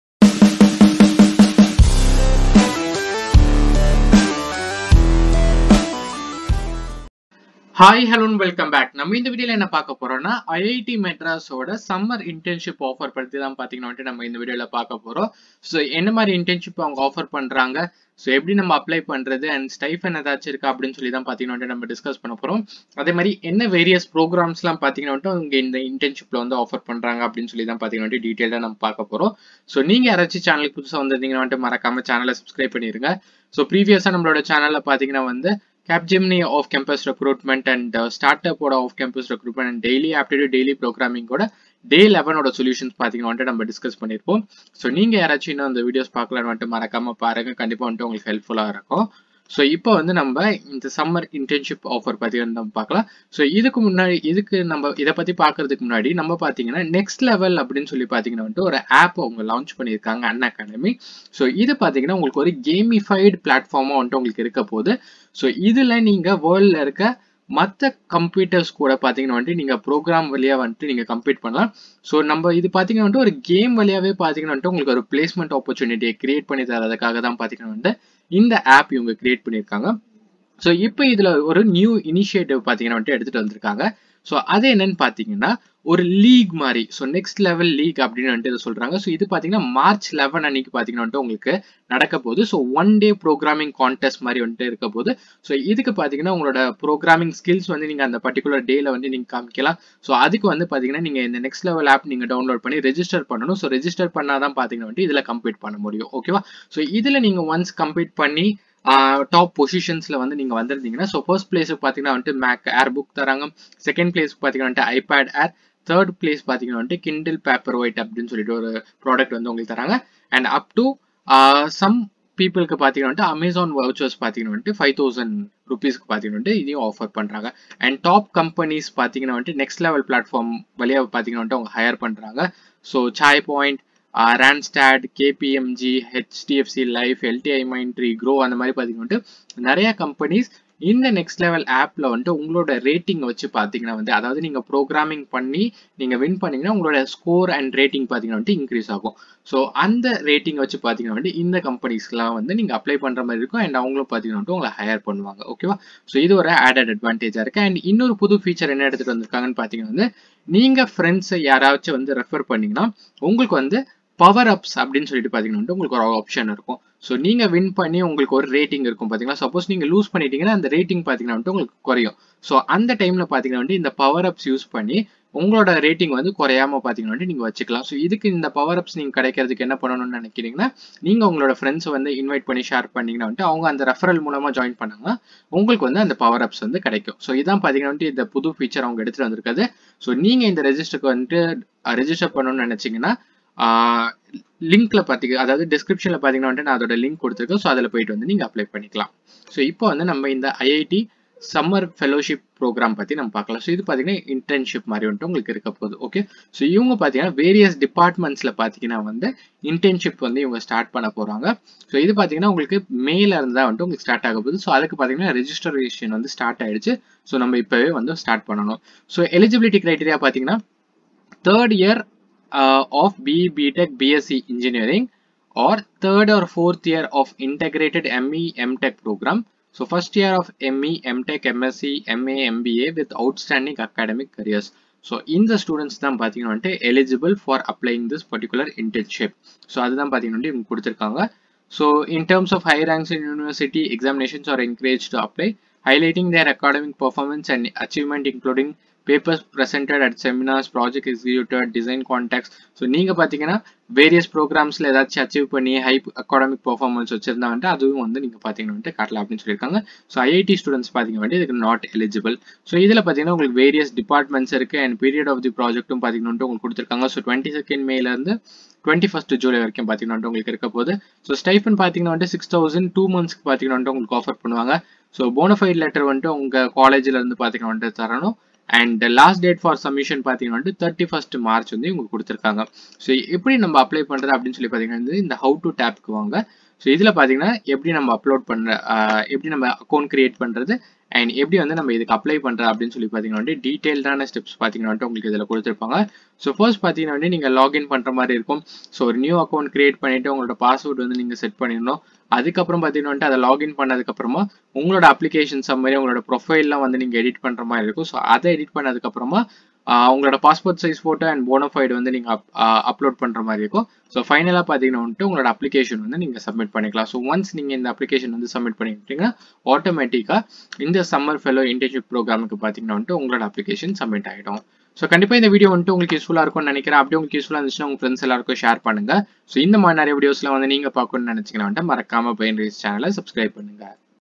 IIT MADRAS Summer Internship 2023 | Anybody Can Apply | BiNaRiEs
In this video, I will be discussing with you about the IIT internship.